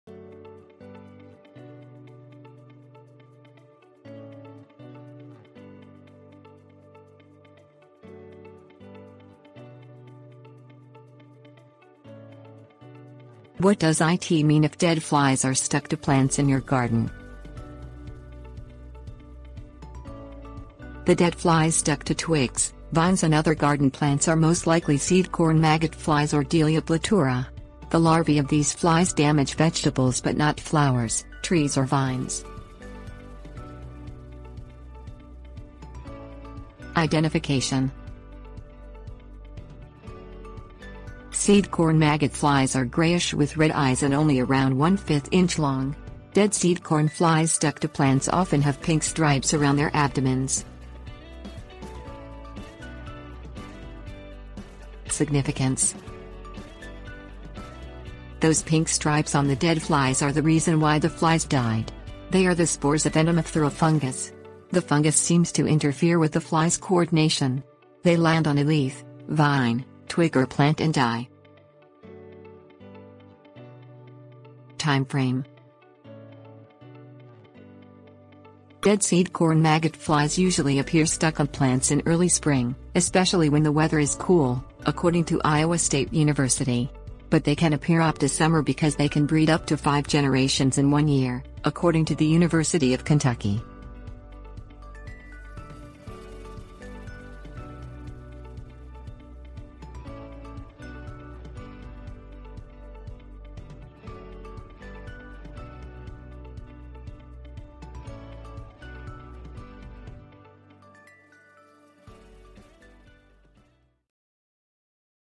What Does it Mean If Dead Flies Are Stuck to Plants in Your Garden?
What Does it Mean If Dead Flies Are Stuck to Plants in Your Garden?. The dead flies stuck to twigs, vines and other garden plants are most likely seedcorn maggot flies or Delia platura. The larvae of these flies damage vegetables but not flowers, trees or vines.

Table of contents What Does it Mean If Dead Flies Are Stuck to Plants in Your Garden?
Identification 00:35
Significance 00:53
Time Frame 01:13